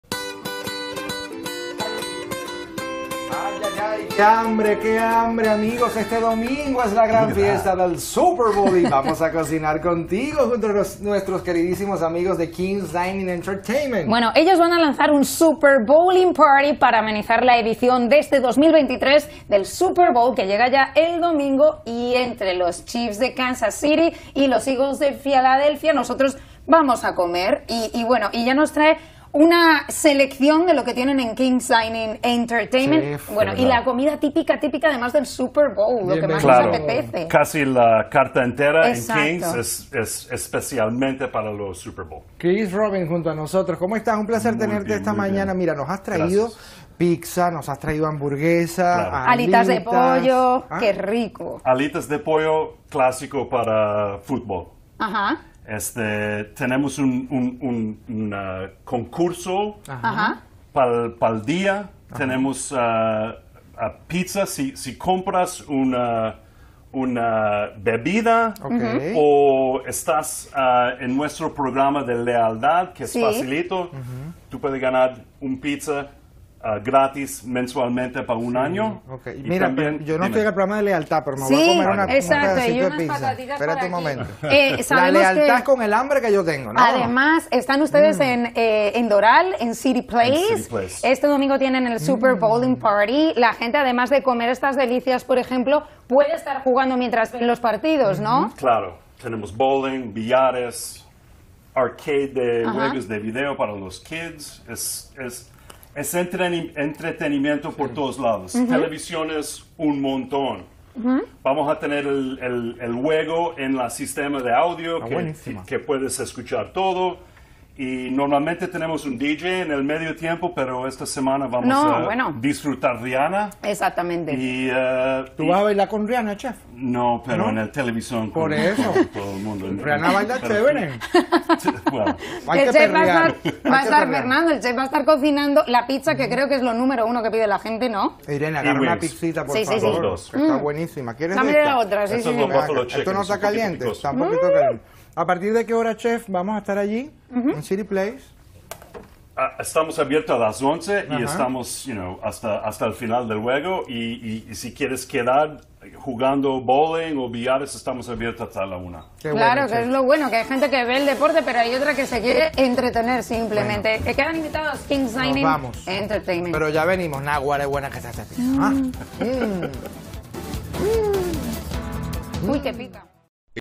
SuperBowling at King's Dining & Entertainment at CityPlace Doral
Plans for The Big Game? King's Dining & Entertainment at CityPlace Doral is hosting a SuperBowling Party on Sunday, Feb. 12, at 6 p.m., so guests can watch The Big Game and enjoy all the entertainment offerings including bowling, billiards, shuffleboard, air hockey, foosball, arcade games and much more.

Enjoy specials including half-priced pizza at halftime, $25 Bud Light buckets, $13 Bud Light and a chance to win a free pizza every month for a year. Telemundo51 shares the details.